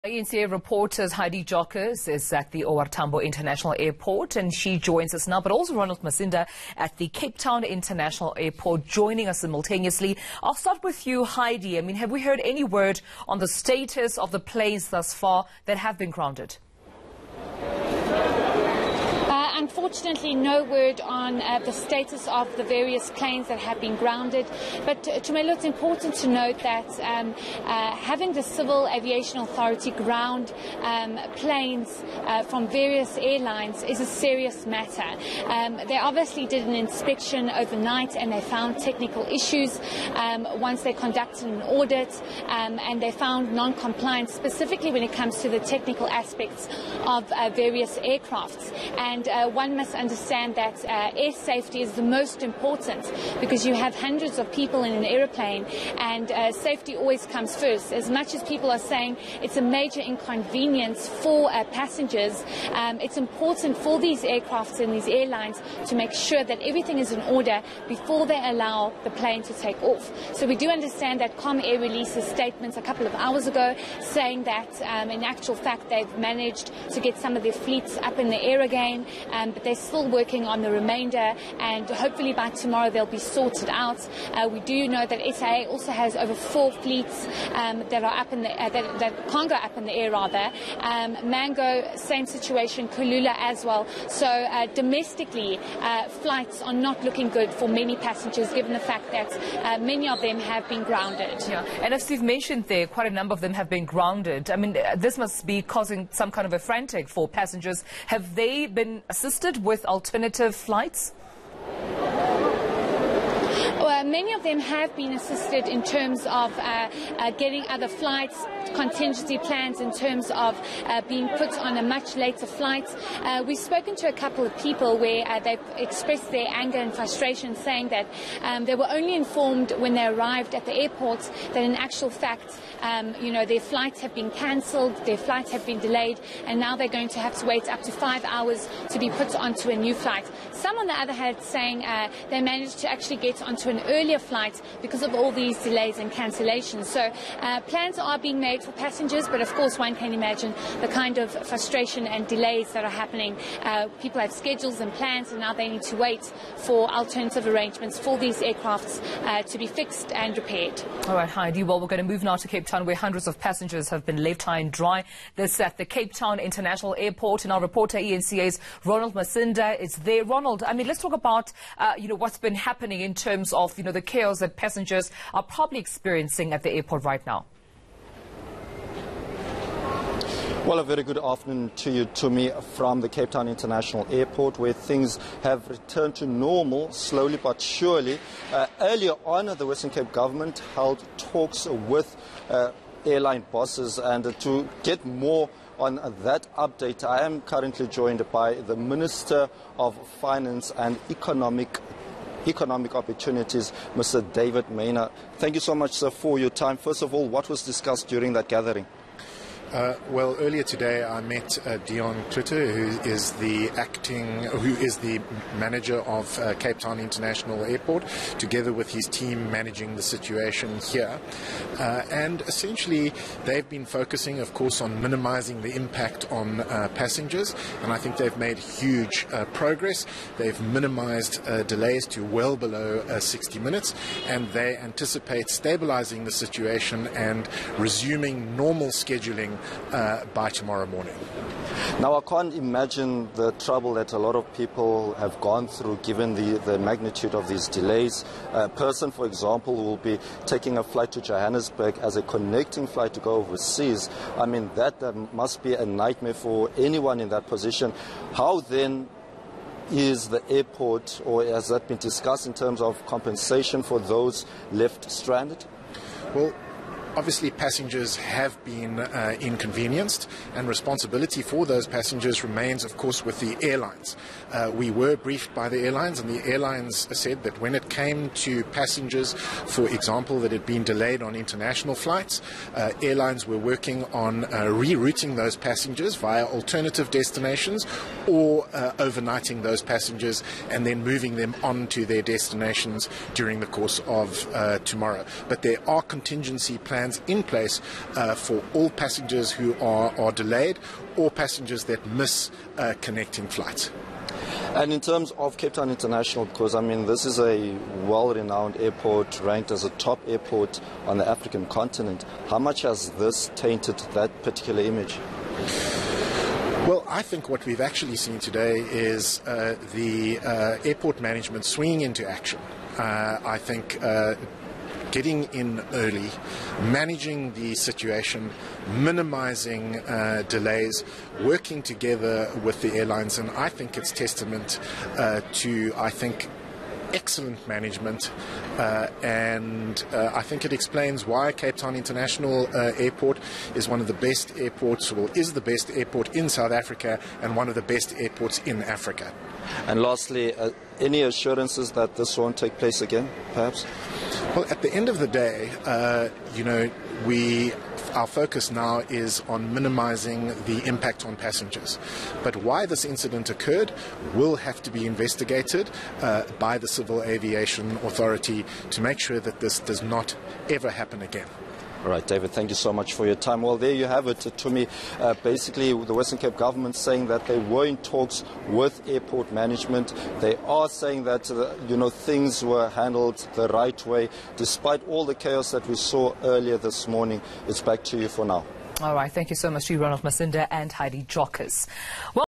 Civil Aviation Authority has grounded planes due to non-compliance
People travelling with SAA, British Airways, Kulula and Comair are being warned of delays. Several flights have been cancelled. Courtesy